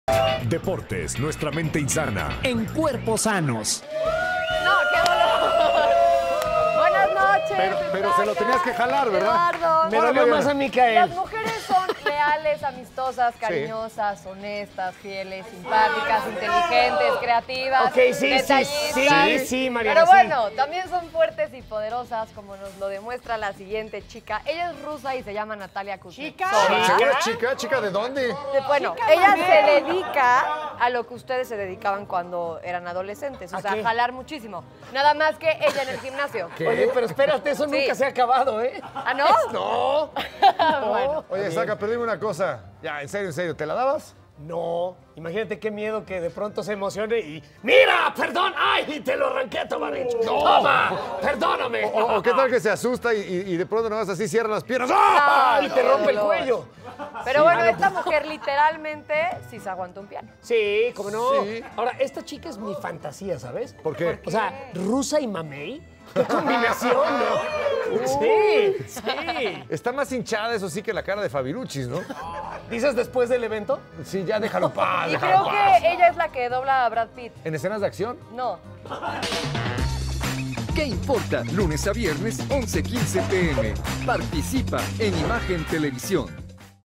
La mujer más musculosa es rusa | Qué Importa
Esta mujer rusa tienen más músculo que toda la producción de Qué Importa.